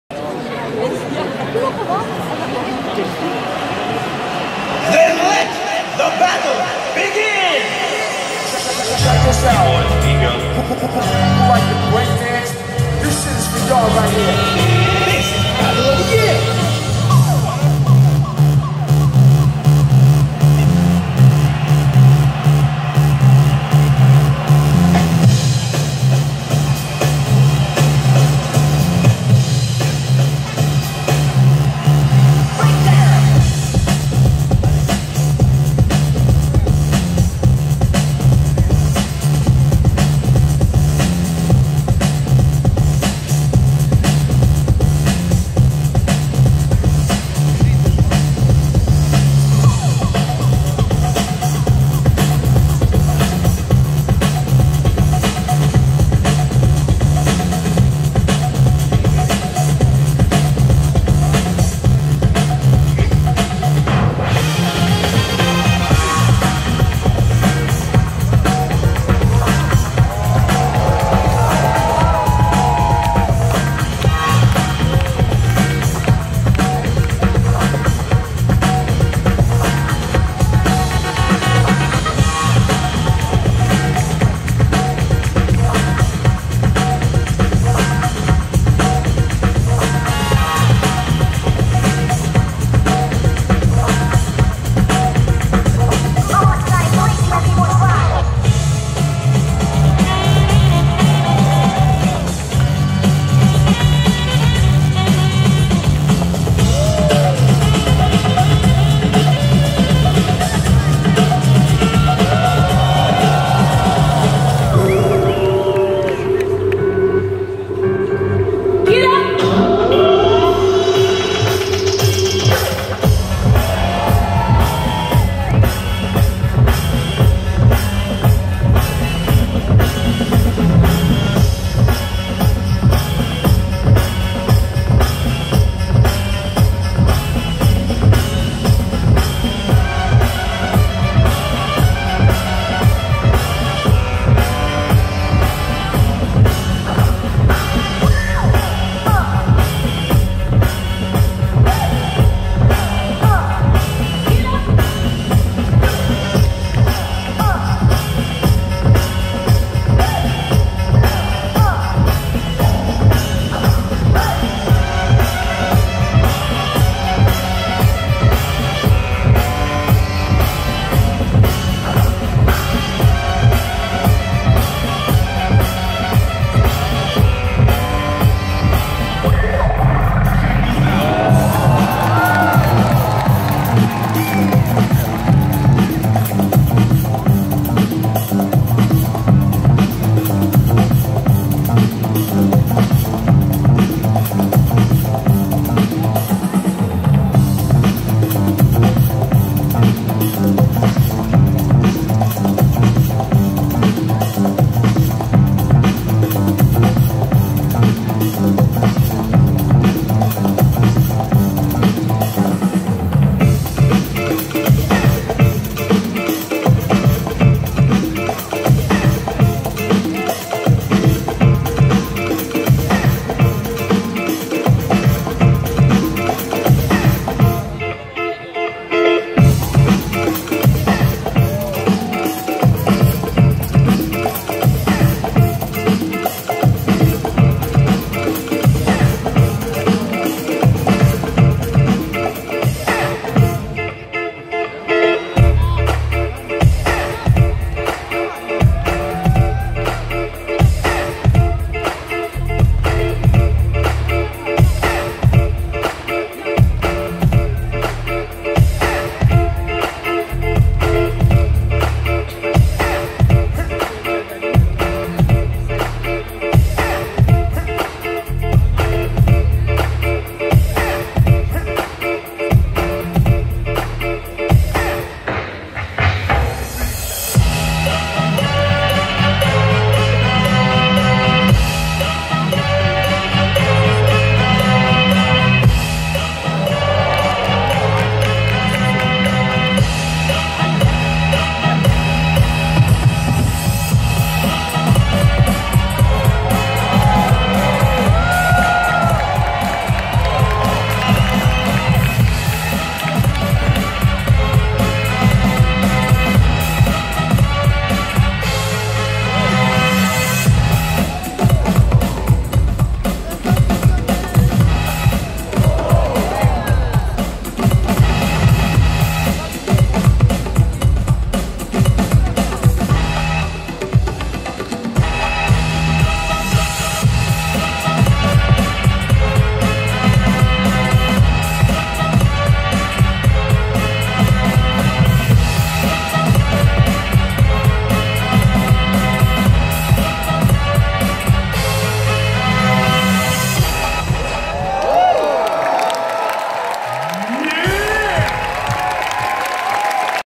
Battle Of The Year International du samedi 21 octobre 2017 vidéo 5
Equipe n°5:Kienjuice Crew (Russie)
Lieu:Grugahalle Essen (Allemagne)